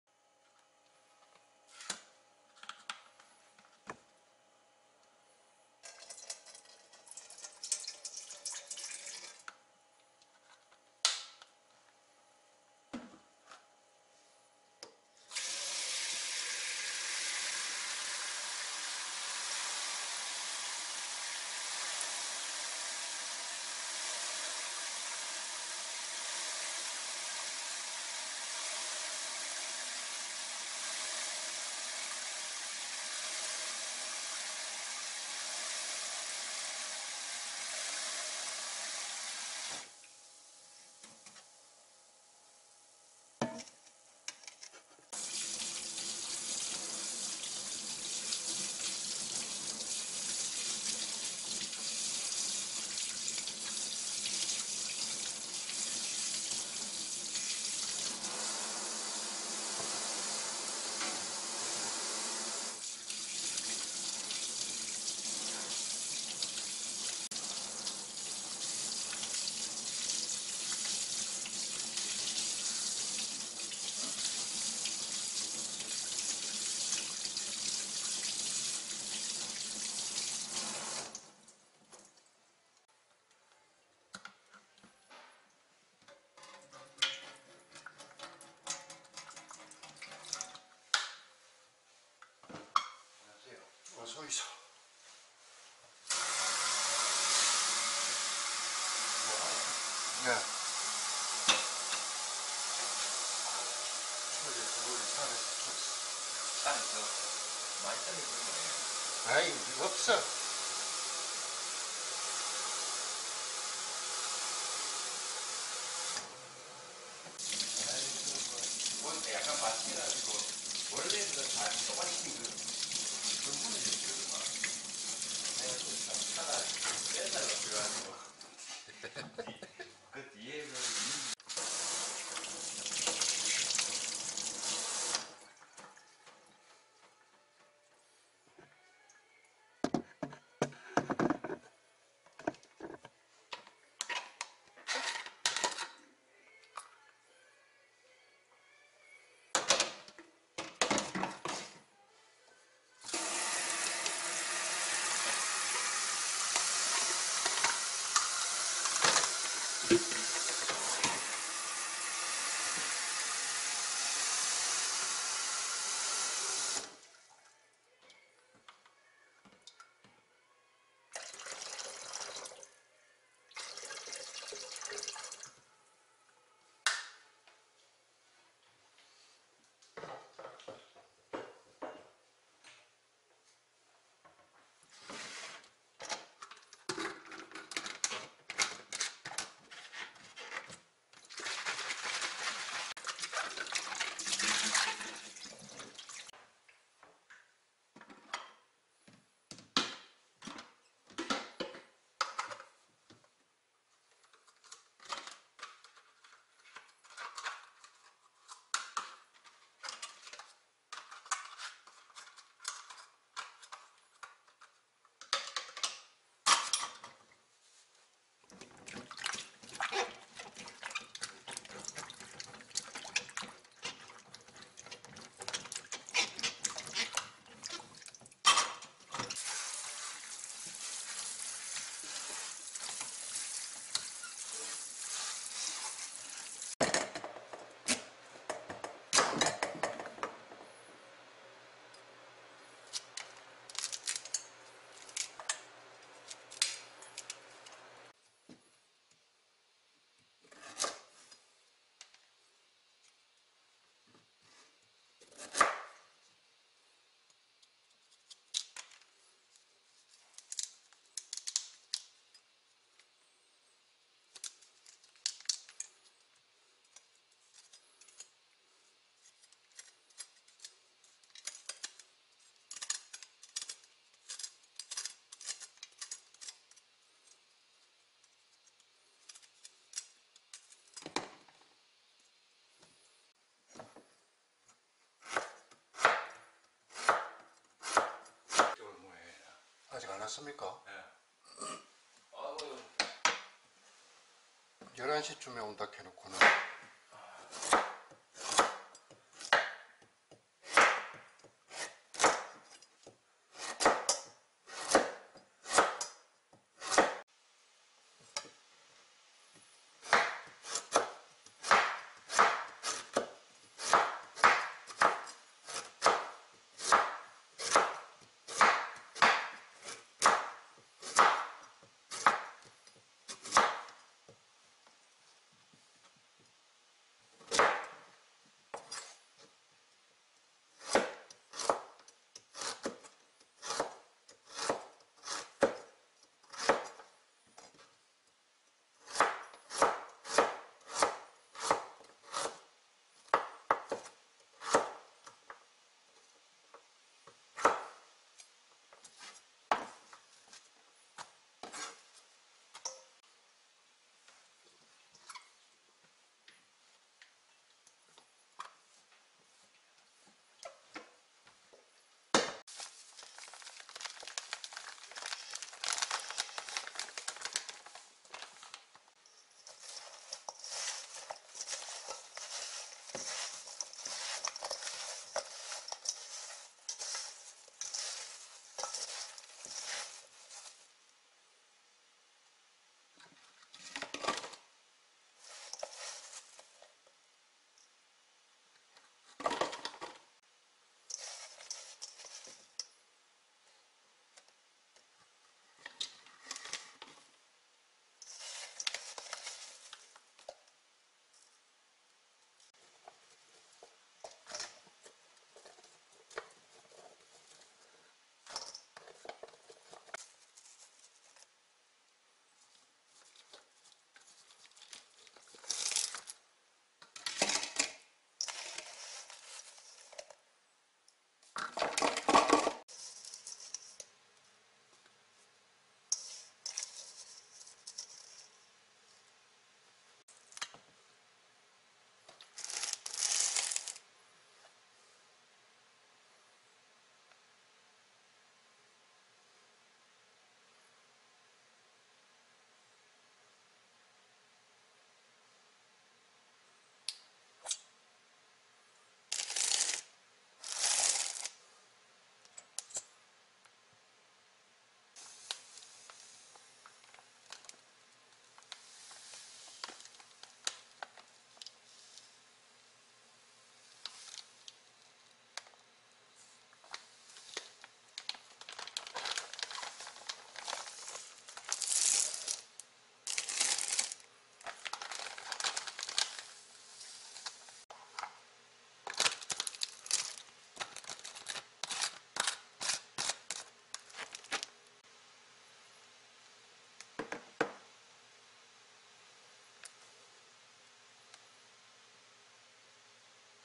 모과청 만들기
근무하는 직장인 도리촌 소유 산에서 수확한 모과로 모과청을 만들어 봤습니다.